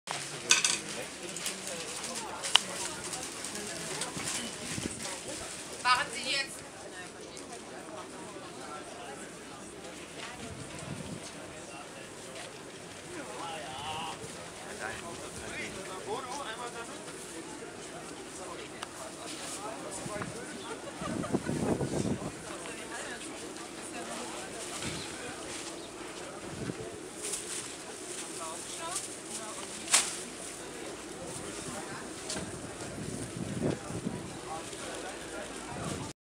Winterfeldtmarkt IV in Berlin Schöneberg
Winterfeldtmarkt auf dem Winterfeldtplatz in Berlin Schöneberg